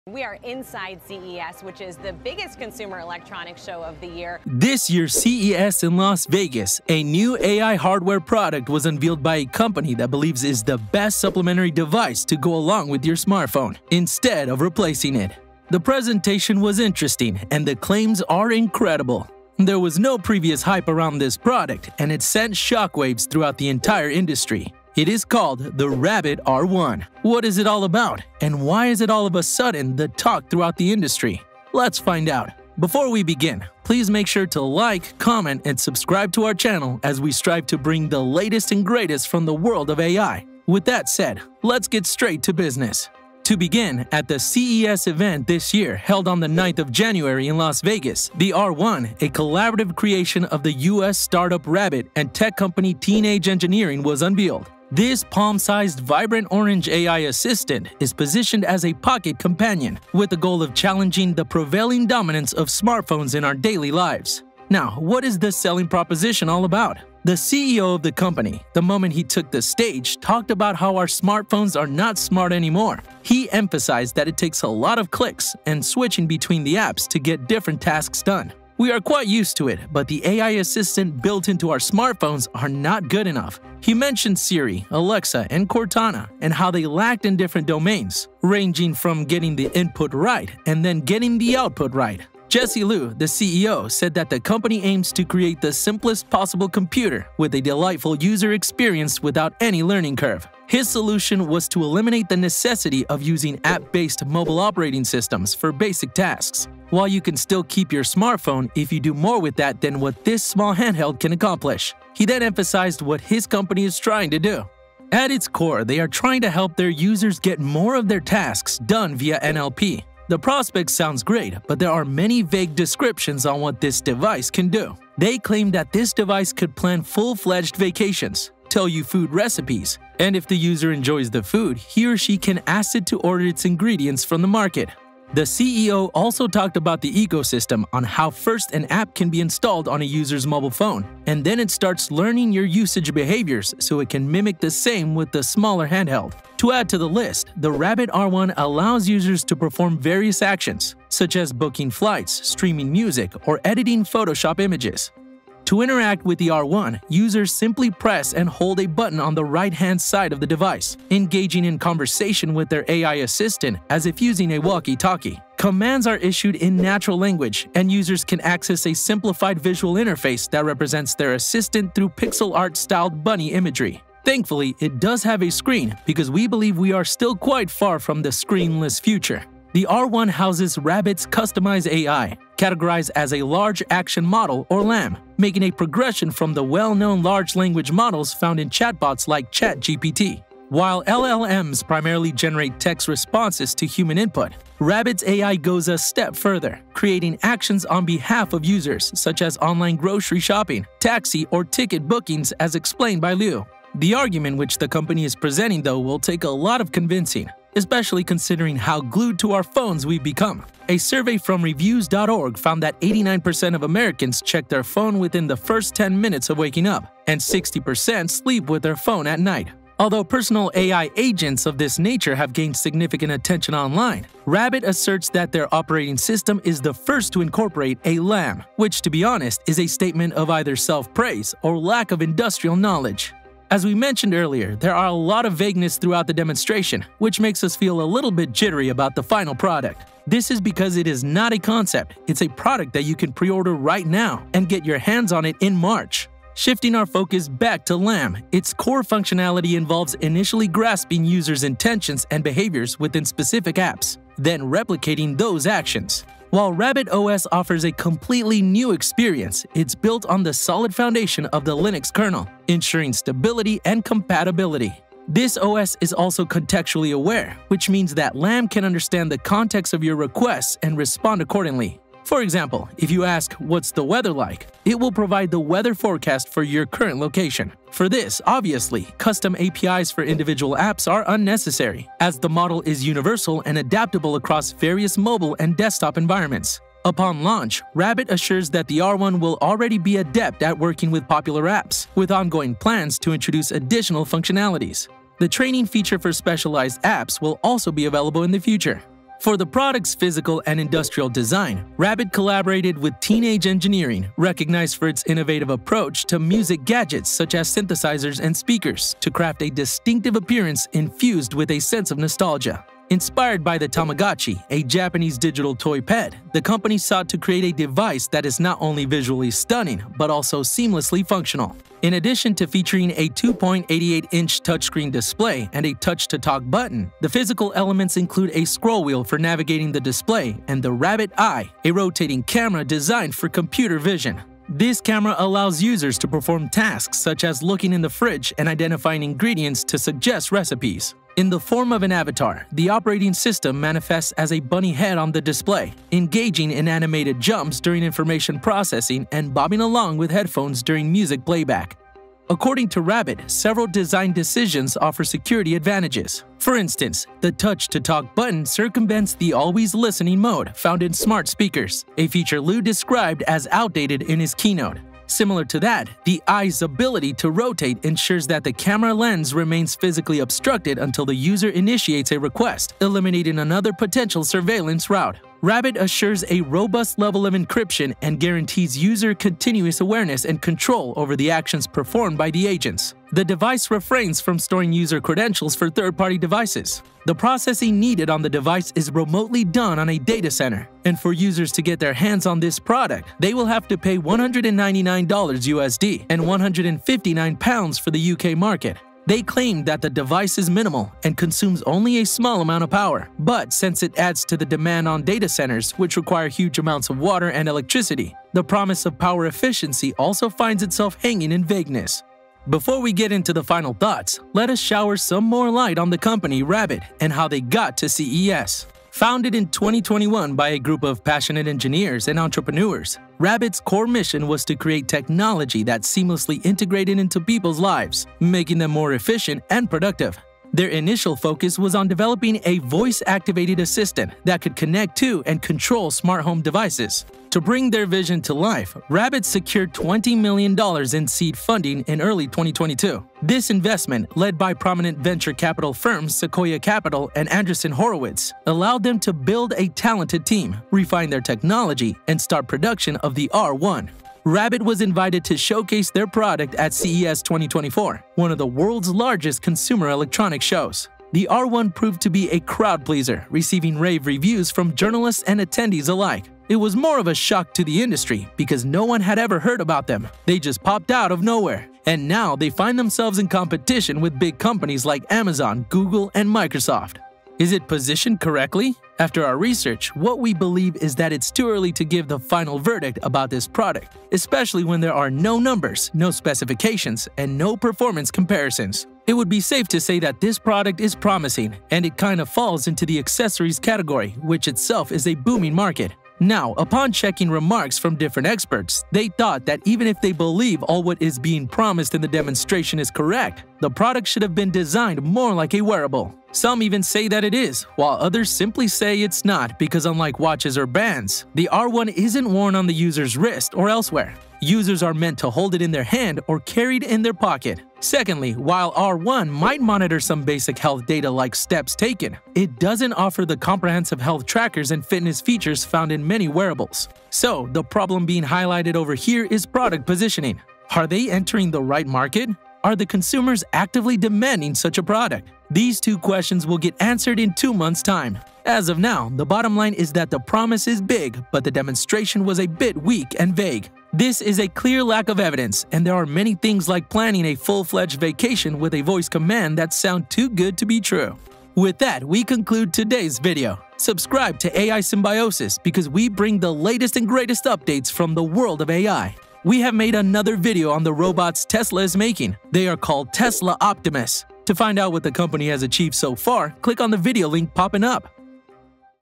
Rabbits New R1 Device Just SHOCKED The Entire AI Agent INDUSTRY
This year’s CES in Las Vegas, a new AI hardware product was unveiled by a company that believes is the best supplementary device to go along with your smartphone, instead of replacing it!
The presentation was interesting, and the claims are incredible. There was no previous hype around this product and it sent shock waves throughout the entire industry
It is called the Rabbit R1!
 What is it all about, and why is it all of a sudden, the talk throughout the industry?

With that said, let’s get straight to business!

To begin, at the CES event this year, held on 9th of January, in Las Vegas, The R1, a collaborative creation of the US start-up Rabbit and tech company Teenage Engineering, was unveiled!
This palm-sized, vibrant orange AI assistant is positioned as a "pocket companion" with the goal of challenging the prevailing dominance of smartphones in our daily lives.
Now what is the selling proposition all about?
The CEO of the company, the moment he took the stage, talked about how our smartphones are not smart anymore!
He emphasized that it takes a lot of clicks, and switching between the apps to get different tasks done!
We are quite used to it but the AI assistants built into our smartphones are not good enough.
He mentioned Siri, Alexa, and Cortana, and how they lacked in different domains, ranging from getting the input right, and then getting the output right!

00:00 Intro
00:44 The Selling Proposition
01:55 Use Cases
02:35 The Works
04:58 Form Factor & Design
06:51 Company’s Journey
07:57 Is it Positioned Correctly?